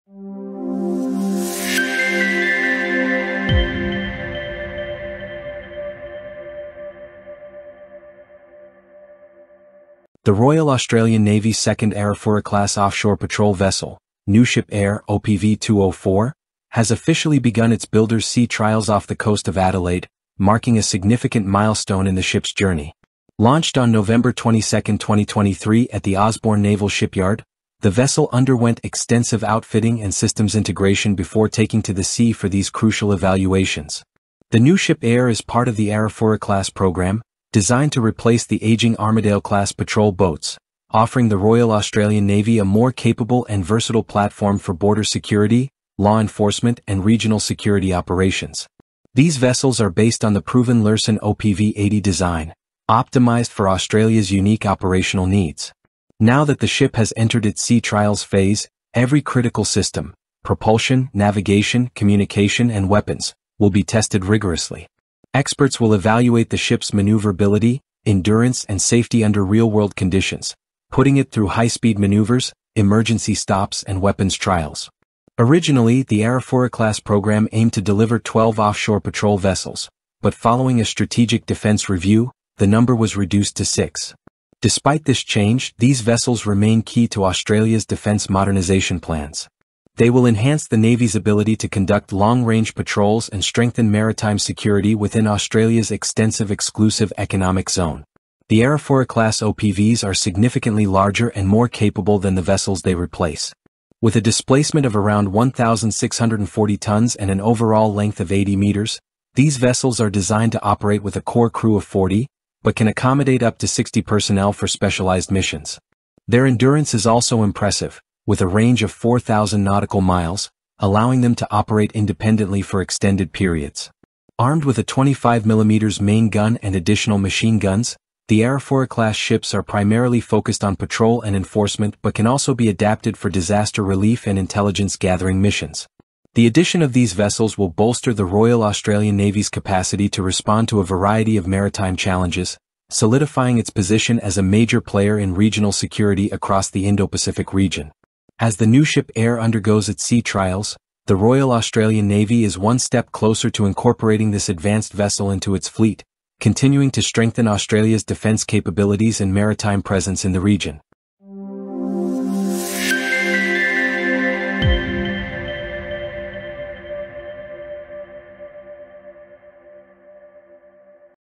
A New Era for Maritime Security Royal Australian Navy's NUSHIP Eyre Begins Sea Trials
The Royal Australian Navy’s NUSHIP Eyre, the second Arafura-class offshore patrol vessel, begins builder’s sea trials off Adelaide. Designed to replace the aging Armidale-class boats, the Arafura-class OPVs are built for long-range patrols, border security, and maritime law enforcement. The sea trials test critical systems like propulsion, navigation, and weapons, ensuring the vessel meets high-performance standards. With an impressive range of 4,000 nautical miles and a versatile design, these OPVs enhance Australia’s regional security efforts.